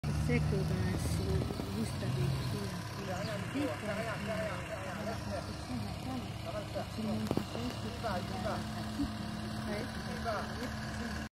Mise à l’eau « le vison » BretagneBateauxBois
Réplique d’un cahotier breton du 18eme siècle. A l’époque transport de cailloux, aujourd’hui bateaux habitable autonome en eau et électricité.